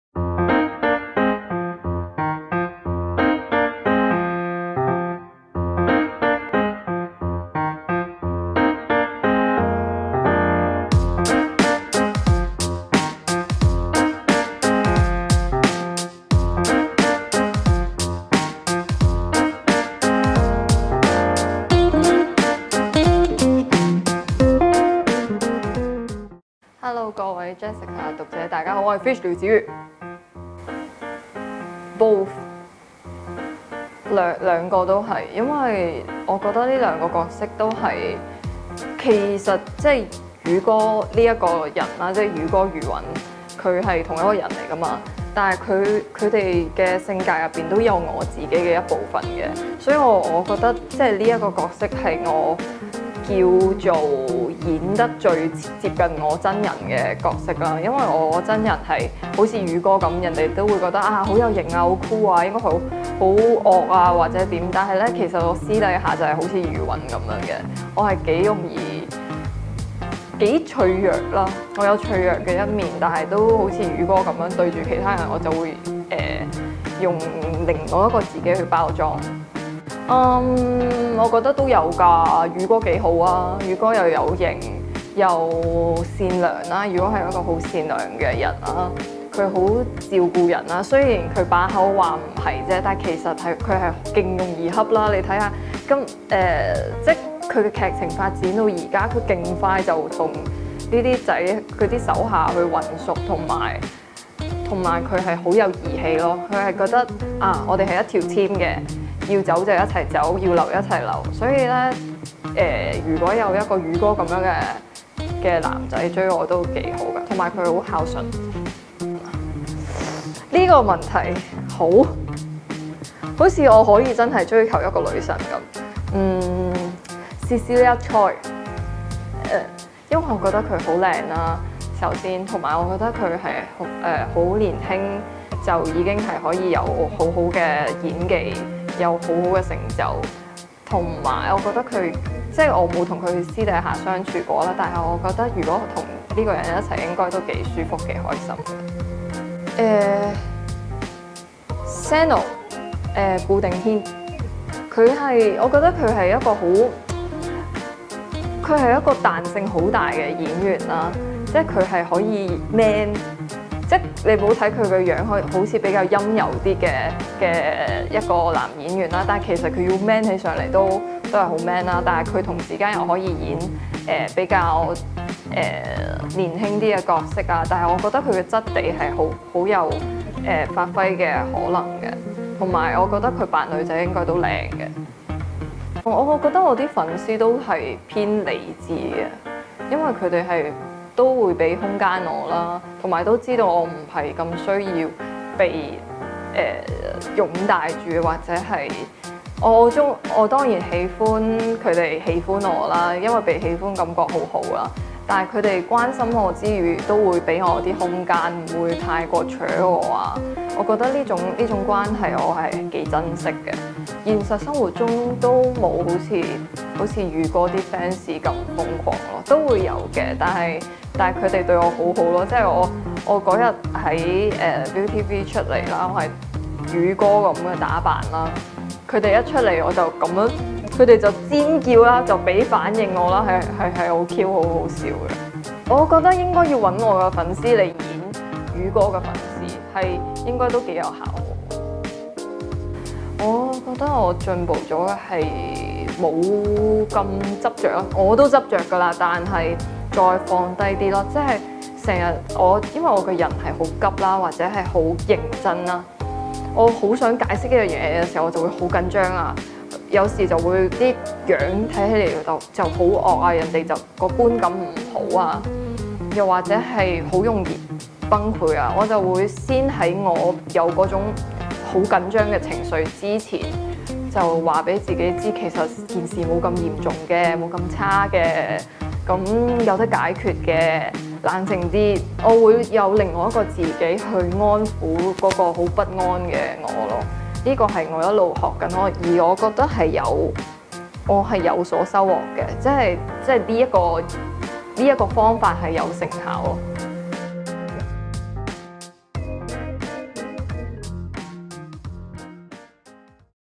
假如廖子妤真係「魚哥」
呢個世界邊有咁多「假如」，所以小編特別鍾意問啲假設性嘅問題……
估唔到「魚哥」上身嘅 Fish 廖子妤 話會放膽追求蔡思韵 (Cecilia Choi)，真係完全唔放劉俊謙在眼內 !? 🤣